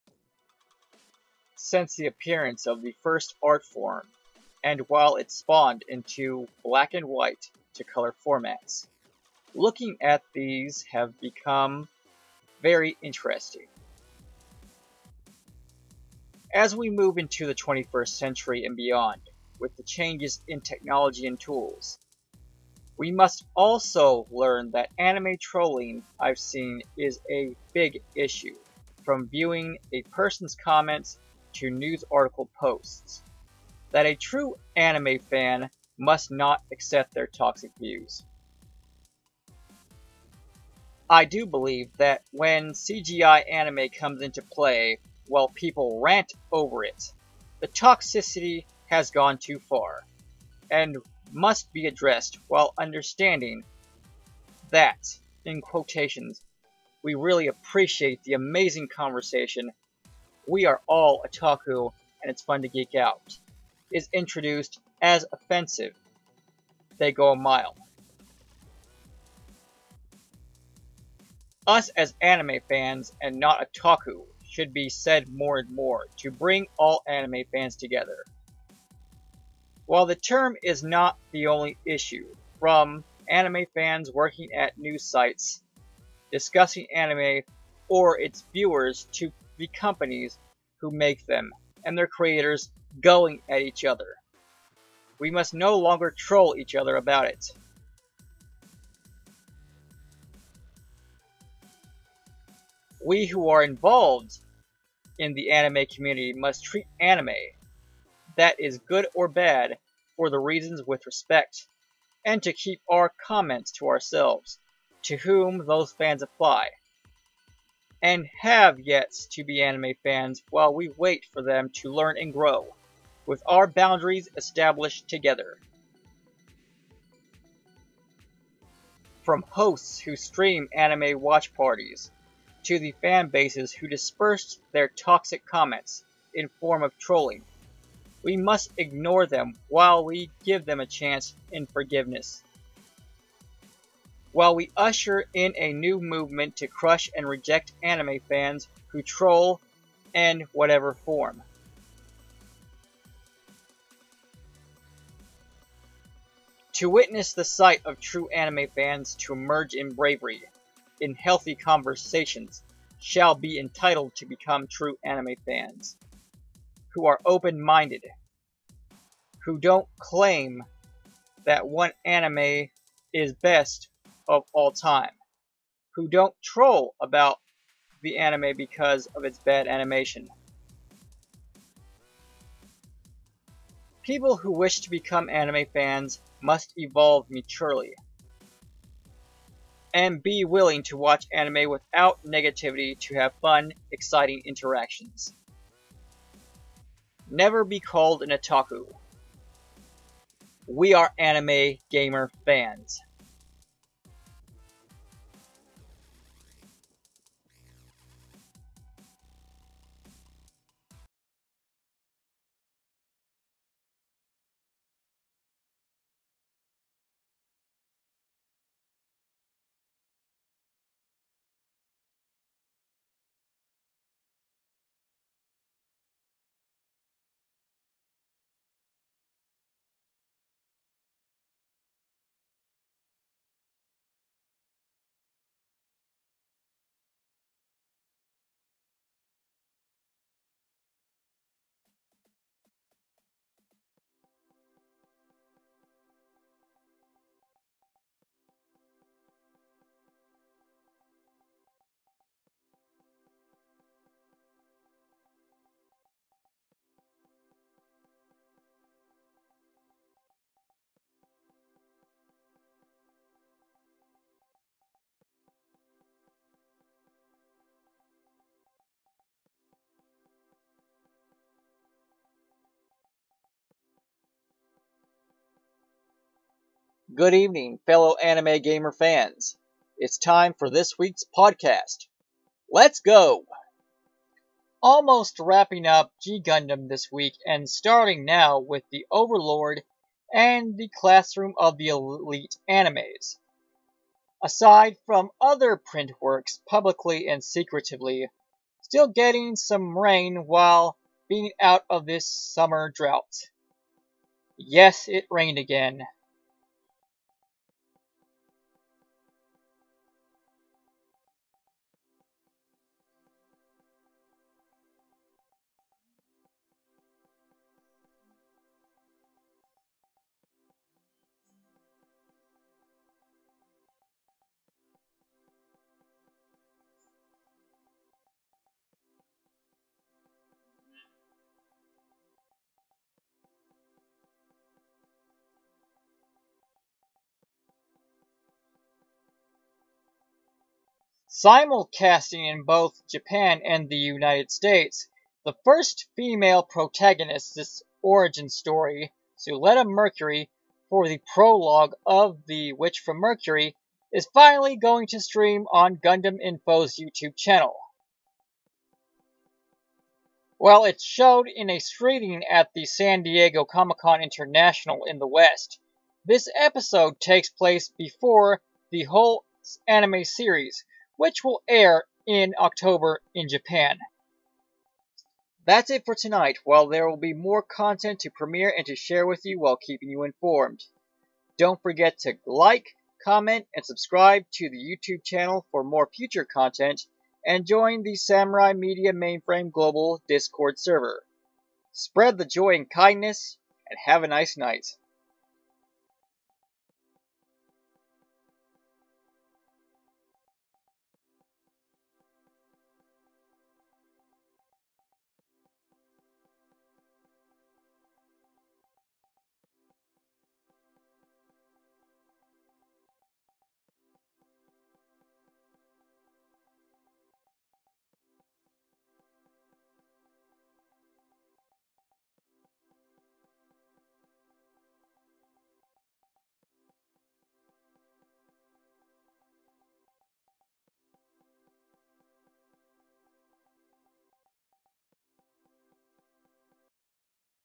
THE WITCH FROM MERCURY PROLOGUE PREMIERES TOMORROW
THANK YOU FOR BEING GOOD LISTENERS OR FEEDBACK. STAY SAFE. STAY POSITIVE AND THINK POSITIVE!

Wrapping up some watched animes, and who's ready to watch The Witch From Mercury Prologue?

Note: The Prologue in CDT will premiere August 31st 2022.

Source

Gundam: The Witch from Mercury Prologue Will Air in September 2022

Come Hang out at Discord!